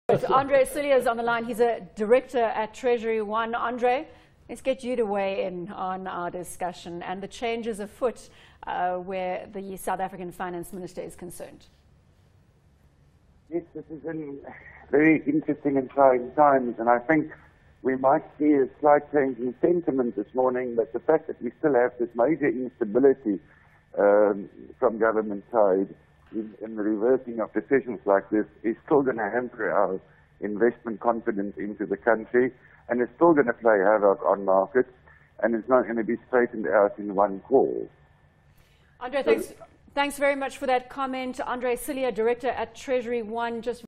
What S.A needs to boost investor confidence?
Original Publish Date: || Mon, 14 Dec 2015 06:41:02 GMT || Andre Cilliers, Director TreasuryOne joins CNBC Africa to give insight on measures required to maintain economic stability and investor confidence.